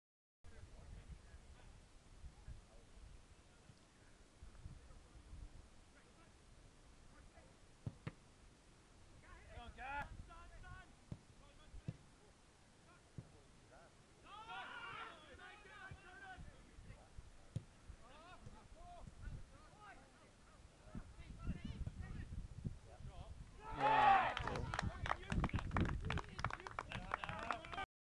Duchy Div2 Game ,Week St Mary 3-2 St Stephen Res
Match highlights,3 minutes into the 2nd half Aled Thomas scores from 25 yards now 1-1